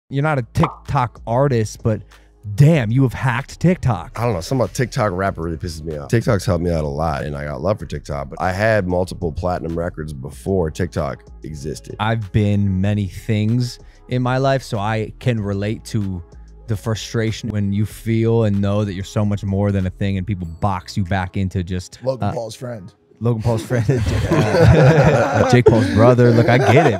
😡 WHAT PISSES OFF YUNG GRAVY
Short from Ep. 361 Yung Gravy On Hooking Up w/ Addison Rae’s Mom, Lisa Ann & Elon Musk Trashing Logan